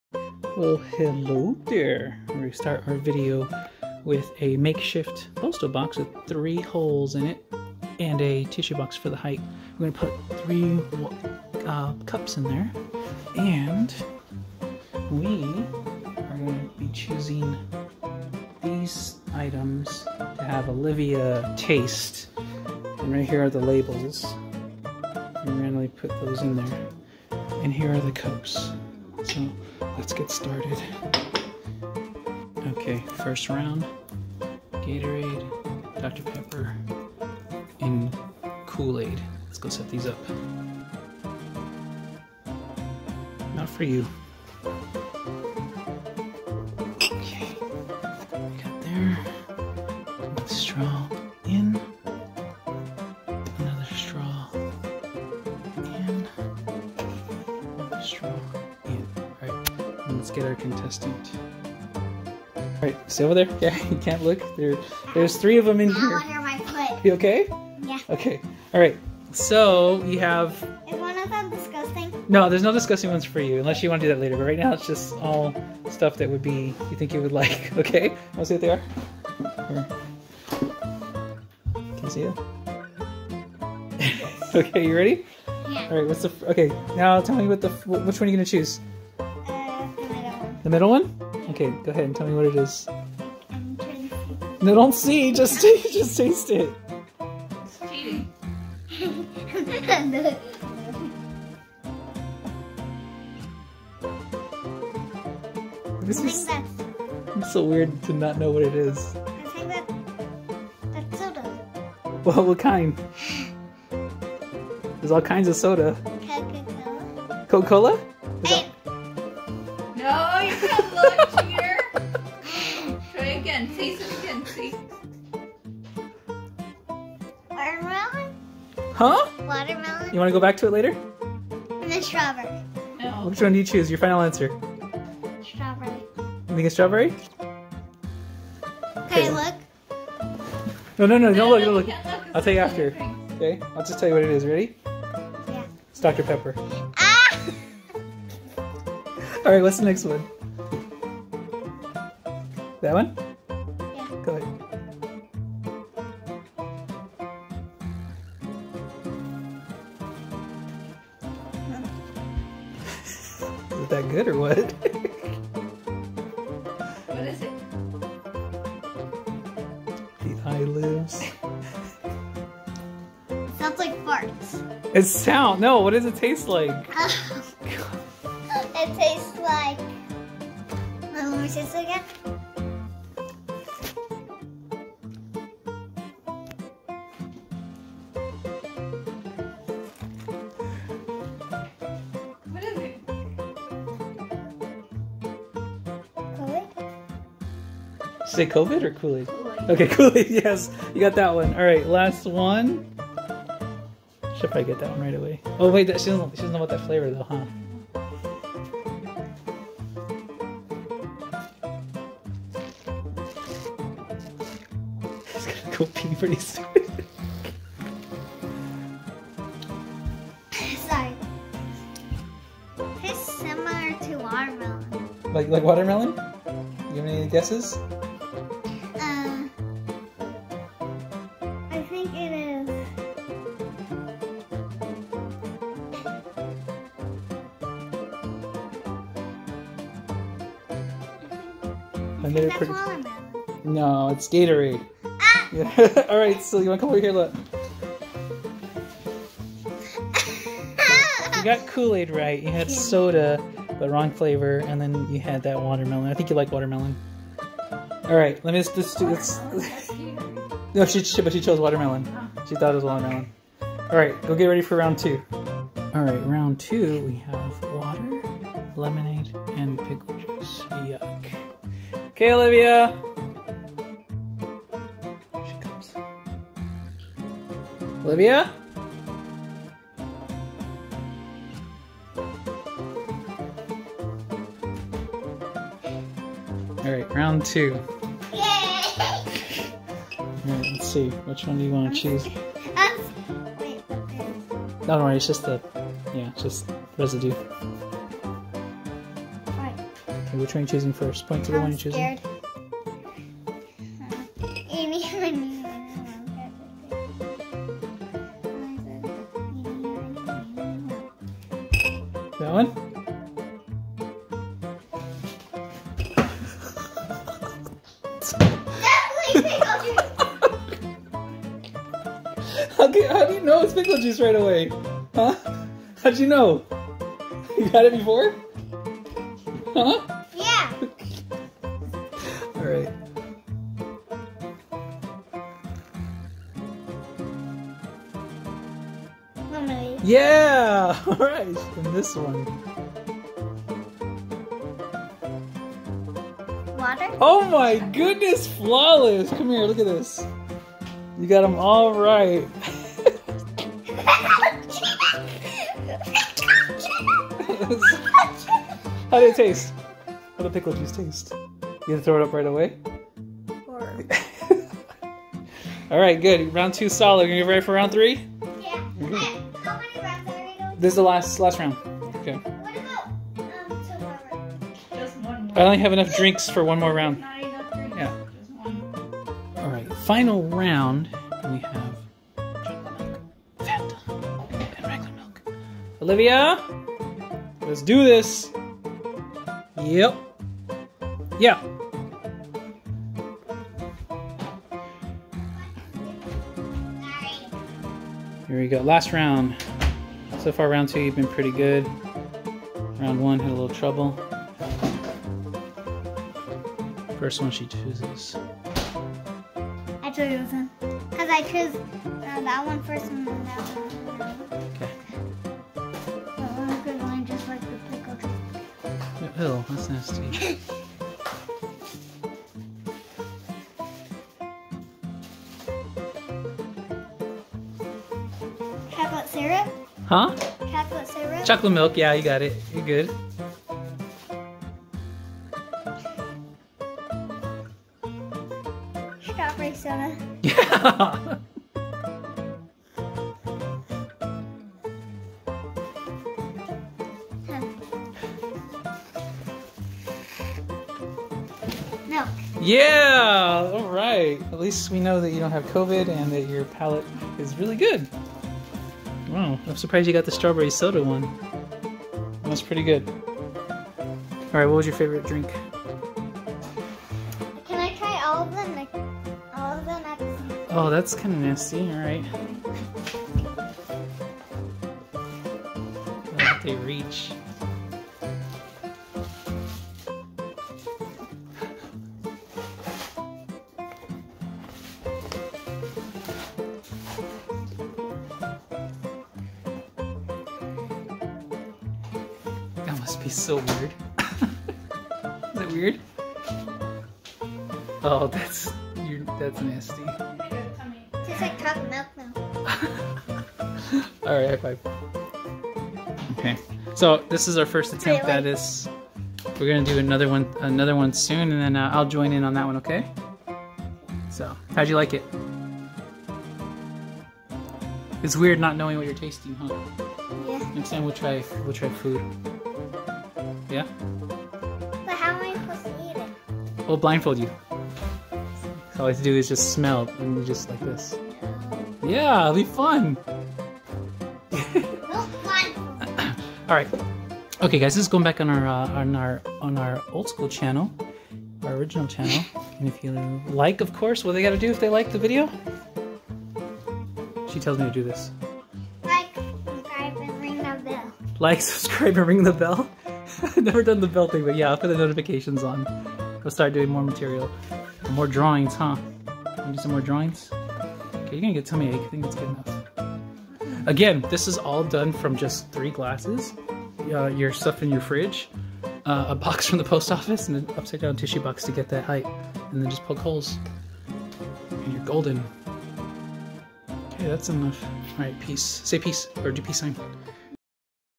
Mystery Drink Challenge Olivia does the Blind surprise drink game-Disney Dad Peter-One Gross Drink
Join Olivia as see tries to do the Mystery Drink challenge with one gross Drink thrown into the mix.